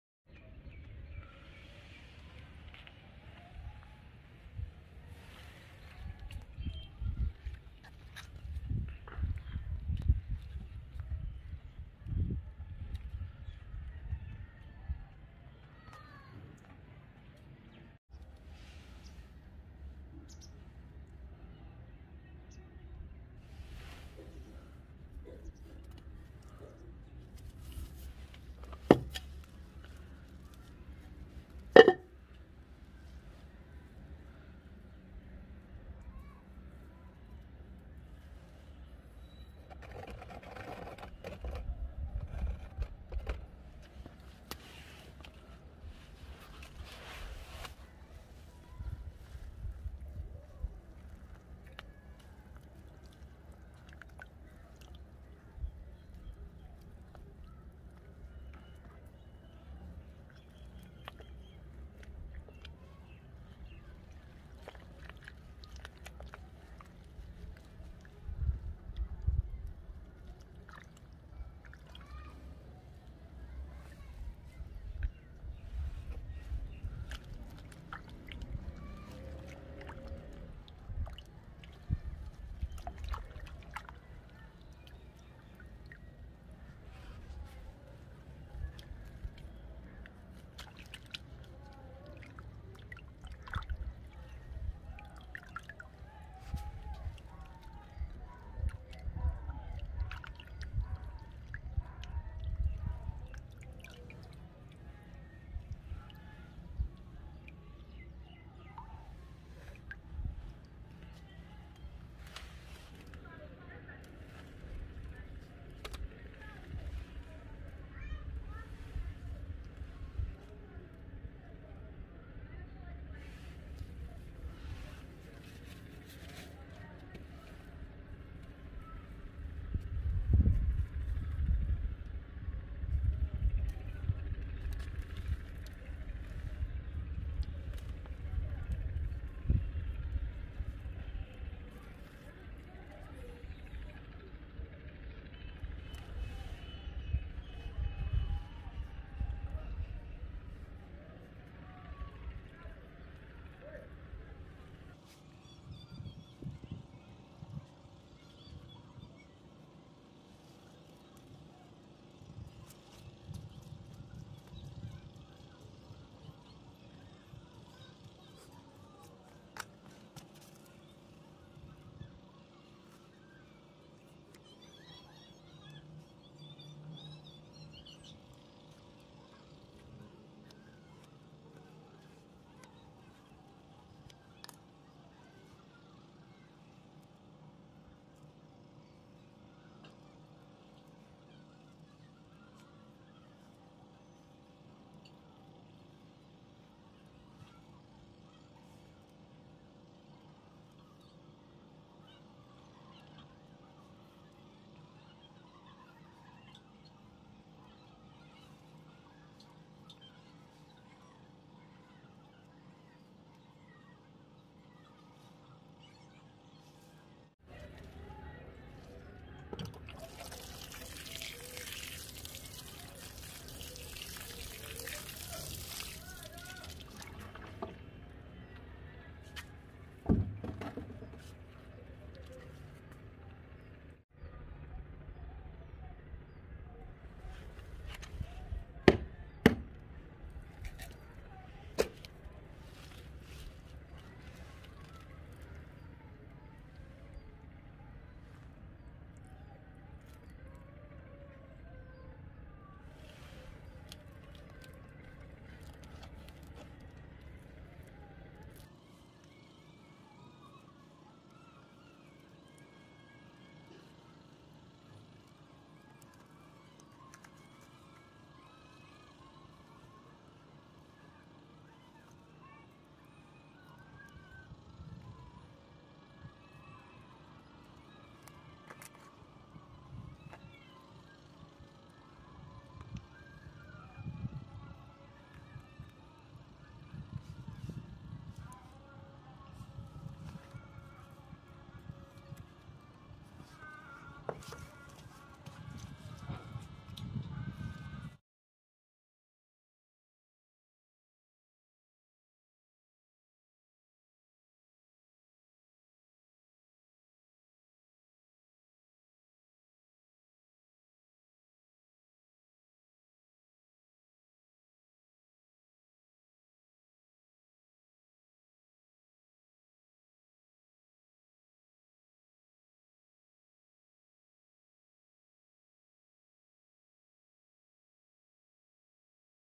Winter Season flowers plant repoting
About this video:  learn how to report seasonal flowers plant paneeri pots/baskets.